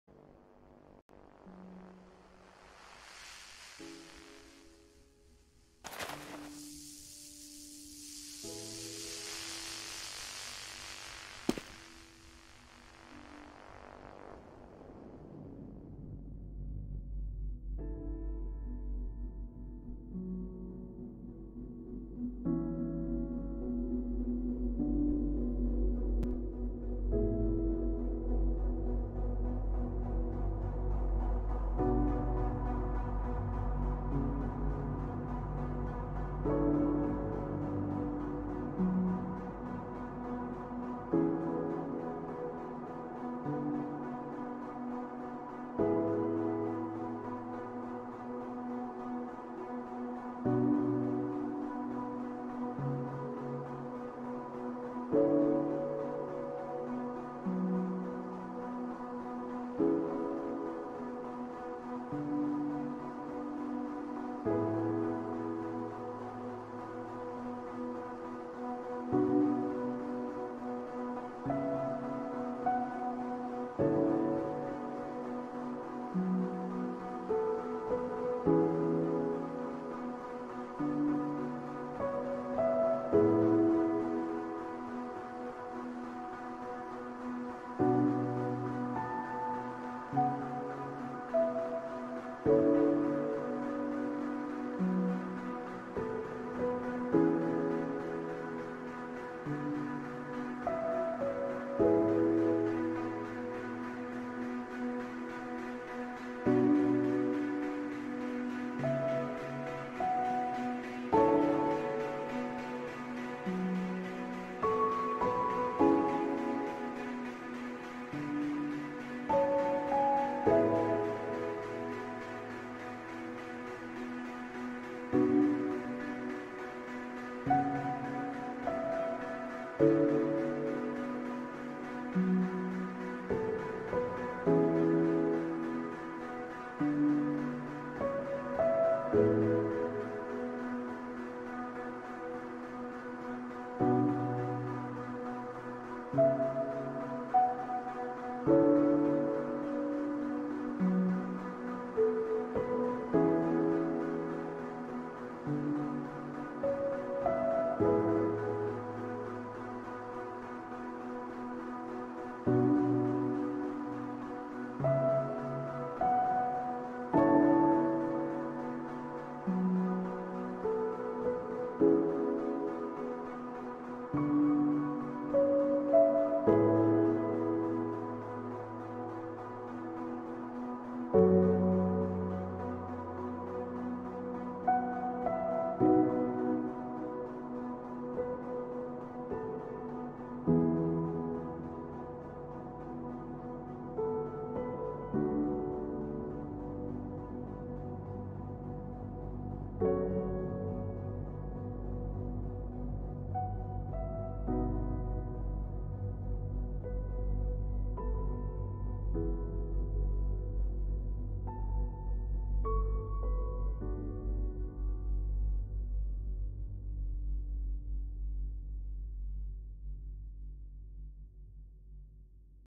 The Art of Relief - br_superflat_v5
Filming this video was honestly rather emotional for me. The Sheer power of the music this map has is amazing, and I would gladly listen to this over and over if I could. It really does make me think deeply about a few things, but I won't go into that here. I hope you enjoy the video :)